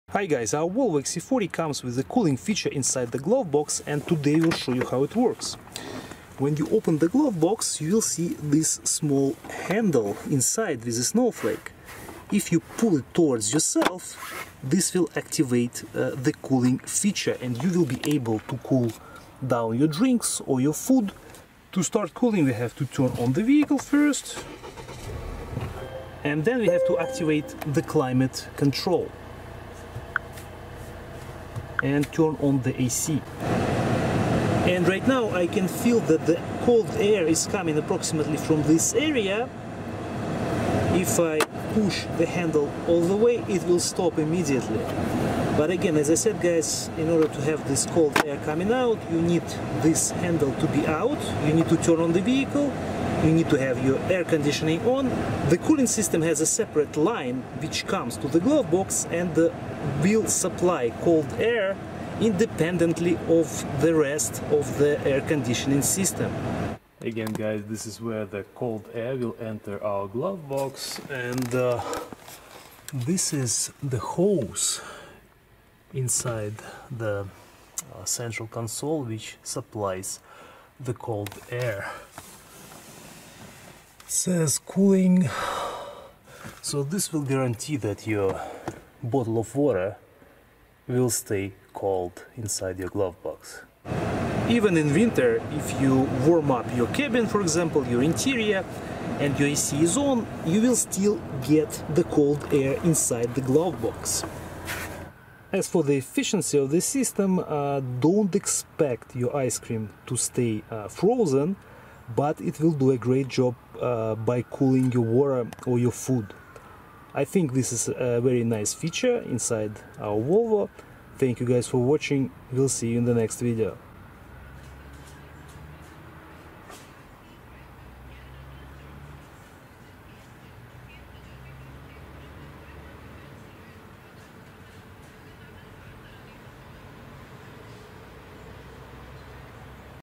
Volvo XC40 (2019-2025): Glovebox Cooler. How Does It Work? XC40 Tips & Tricks!
XC40 Tips & Tricks! This video will show the location and the operation of the cooler inside the glovebox of the 2019, 2020, 2021, 2022, 2023, 2024, and 2025 Volvo XC40 (EX40).

In order to activate the glovebox cooler, you need to pull the handle with the snowflake inside the glovebox towards yourself. The climate control should be active and air conditioning should be on as well.

Camera Used To Record This Video: Panasonic HC-WXF1 4K Cinema-like Camcorder, 24x Leica Dicomar Lens, 1/2.5" Bsi Sensor, Three O.I.S. Stabilizer Systems